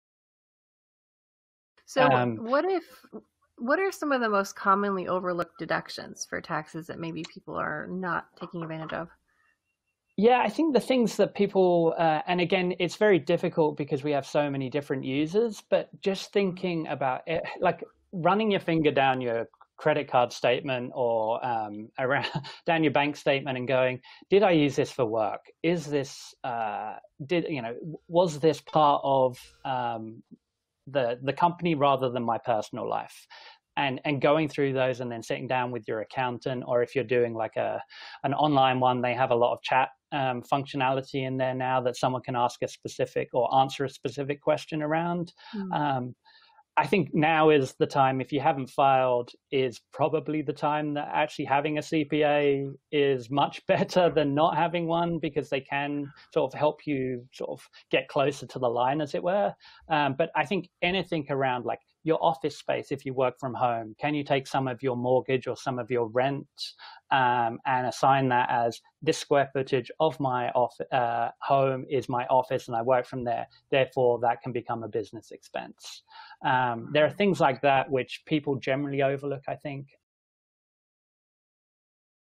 Survive & Thrive - Overlooked Tax Deductions
Overlooked tax deductions can sneak up on you - especially during a crisis. Learn from Tailwind's financial director, Andy, to understand how to proactively prepare your business for financial success!